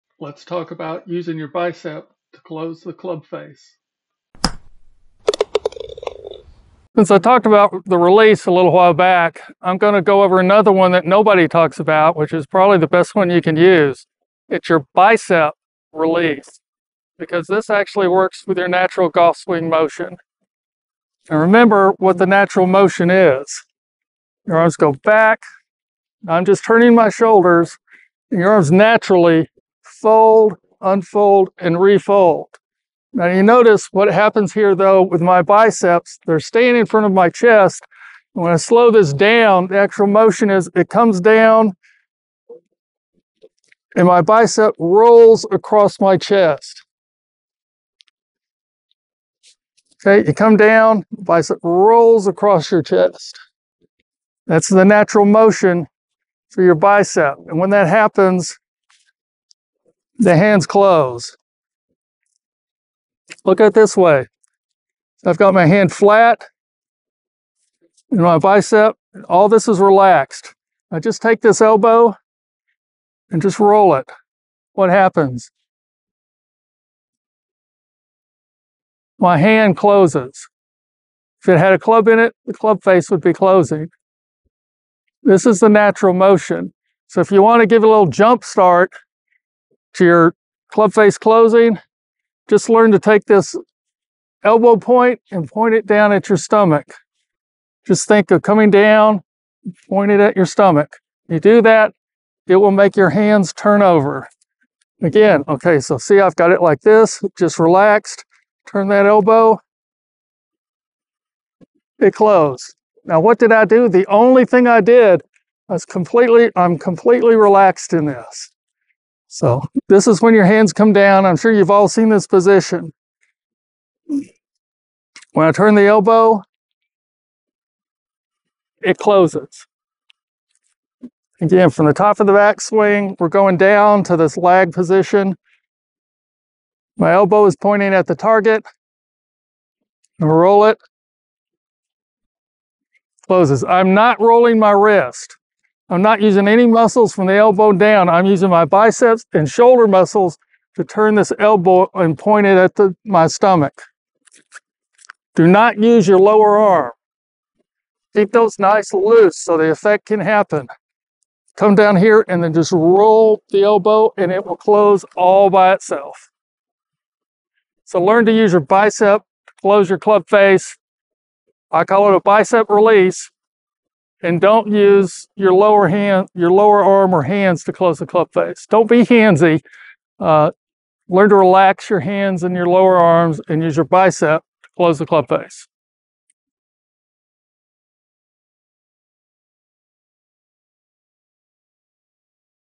Full Video: What is the Bicep Release? ep41
Title: Master Your Swing: The Bicep Release Technique for Club Face Control

Description:
In this enlightening video, we introduce you to a game-changing concept in golf swing mechanics: the bicep release technique. Unlike conventional release methods that rely on muscles below the elbow, the bicep release harnesses the natural movement of your arm for superior club face control and consistency.

By using your bicep to close the club face, you tap into the body's innate biomechanics, promoting a more efficient and effortless swing motion. This technique ensures a smooth and reliable release of the club, leading to improved accuracy and ball flight.

Join us as we explore the fundamentals of the bicep release, providing step-by-step instructions and practical tips to help you incorporate this technique into your swing. Whether you're a beginner looking to refine your mechanics or an experienced player seeking to optimize your performance, mastering the bicep release can elevate your game to new heights.

Watch now to learn why the bicep release is superior to other release methods in golf and discover how it can transform your ball striking and overall consistency on the course. Let's tee off on the journey to better golf together!